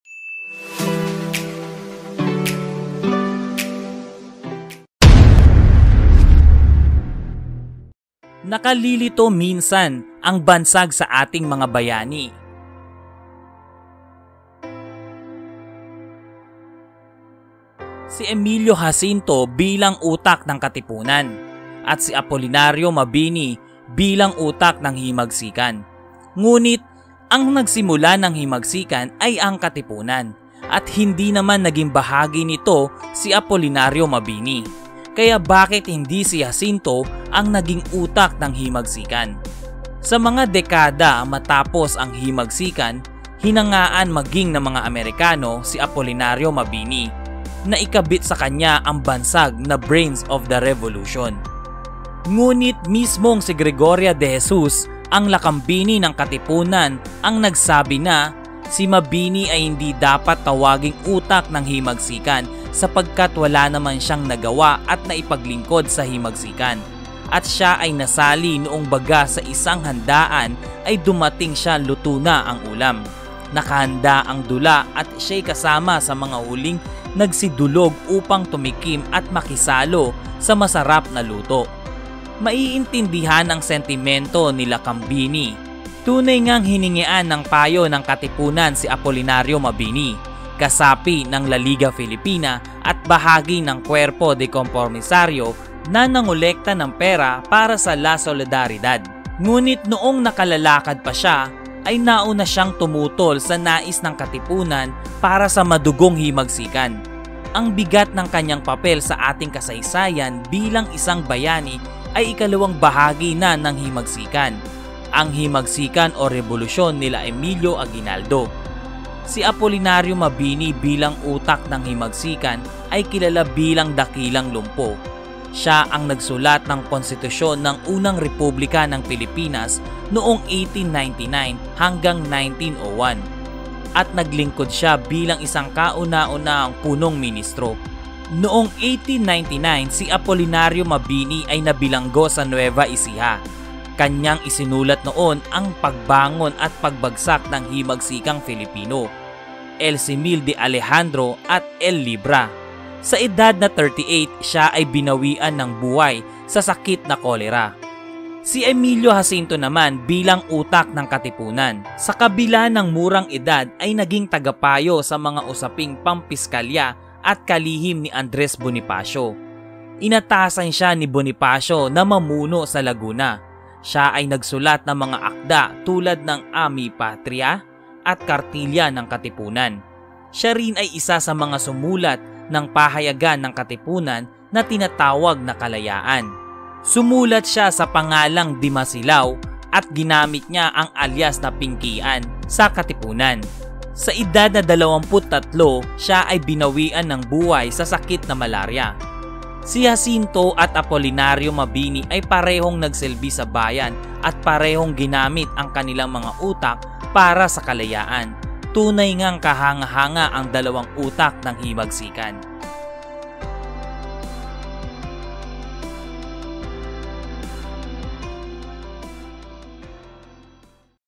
SINO ANG TOTOONG "UTAK": SI MABINI O SI JACINTO? | KATIPUNAN | HIMAGSIKAN 1896
Ang Himagsíkang 1896 ang pambansang paghihimagsik ng mga Filipino laban sa kolonyalismong Español at sumiklab noong Agosto 1896 sa pangunguna ng Katipunan. Itinatag ang Katipunan o Kataastaasang Kagalanggalang na Katipunan ng mga Anak ng Bayan noong gabi ng 7 Hulyo 1892 pagkatapos mapabalita ang pagdakip at pagdestiyero kay Jose Rizal. Inisip ng mga taga pagtatag na sina Andres Bonifacio, Teodoro Plata, Valentin Diaz, Ladislao Diwa, Deodato Arellano, at dalawa pa na walang saysay ang kilusang repormista. Ang Katipunanay isang lihim na samahan para ibagsak ang gobyernong dayuhan. Unti-unti itong kumalat sa mga bayan sa paligid ng Maynila at nagkaroon ng mga sanga hanggang Ilocos sa hilaga, Kabikulan, Aklan, Cebu, Palawan, at hanggang Mindanao bago natuklasan noong Agosto 1896.